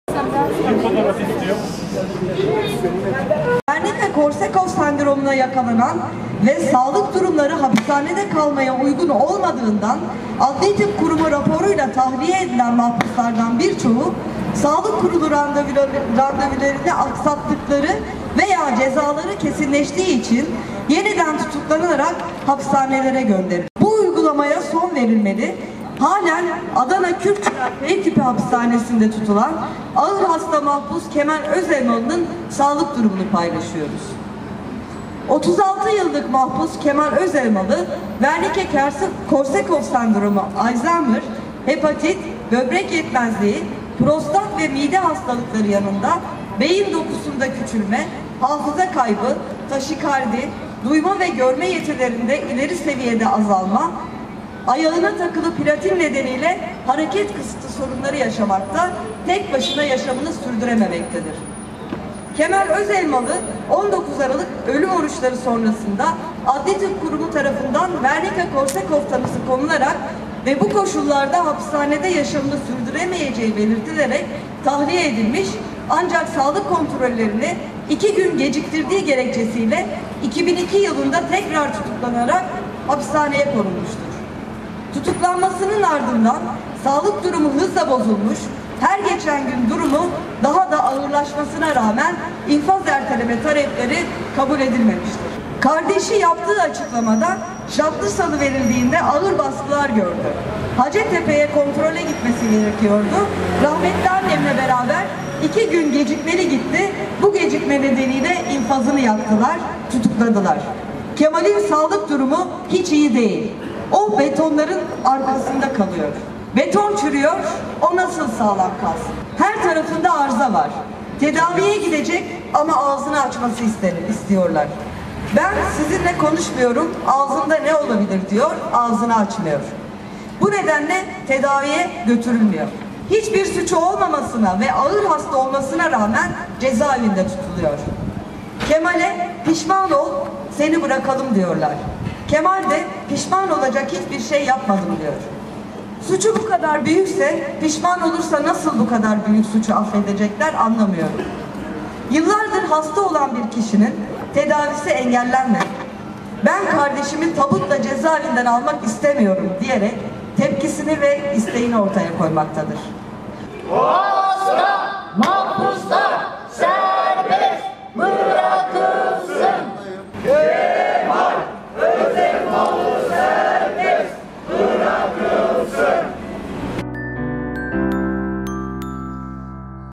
‘F OTURUMU’NDA WERNİCKE KORSAKOFF HASTASI ÖZELMALI İÇİN ÇAĞRI
"F oturumu”nun 638’inci haftasında tek başına yaşamını idame ettiremeyen ağır hasta tutsak Kemal Özelmalı için tahliye ağrısında bulundu.